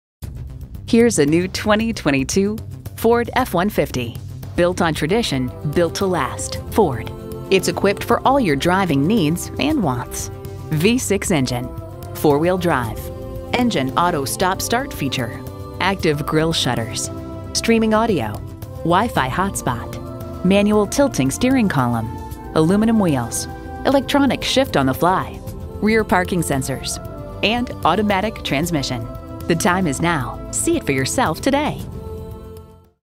New 2022 Ford F-150 Wausau, WI
Year: 2022
Make: Ford
Model: F-150
Trim: XLT
Engine: 2.7L V6 EcoBoost
Transmission: 10-Speed Automatic
Color: White
Interior: Black
Stock #: 2754F
VIN: 1FTEW1EP8NKE81482
2022 Ford F-150 XLT Space White Metallic 2.7L V6 EcoBoost

Address:
2727 North 20th Ave